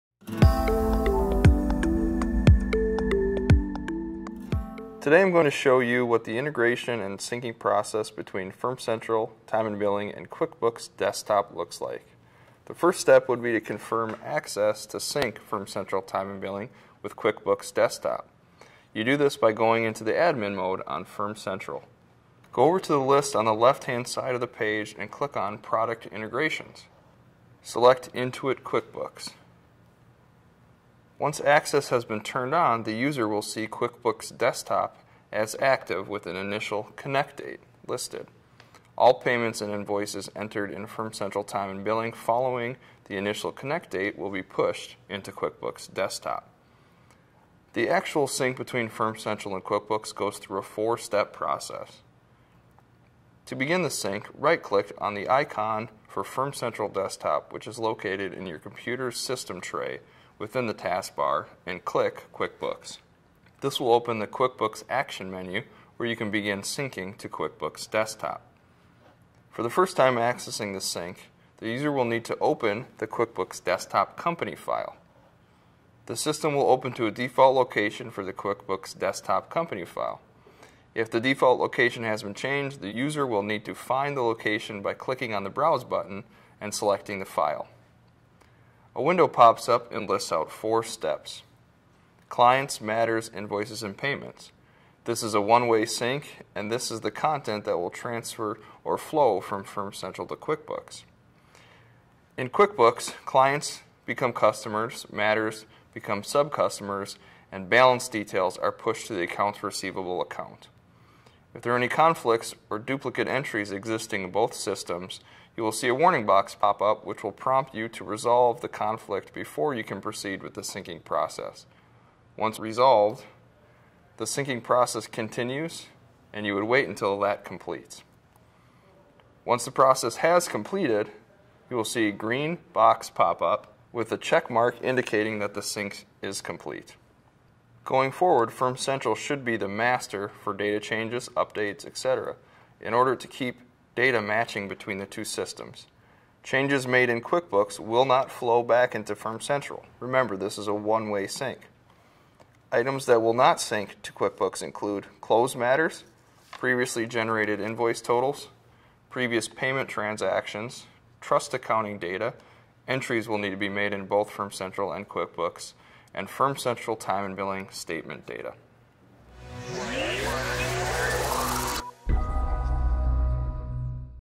Firm Central Time & Billing QuickBooks Desktop Integration
Learn how to connect your Firm Central account with QuickBooks desktop for back office financial reporting.

Learn more about how Firm Central can help with law practice management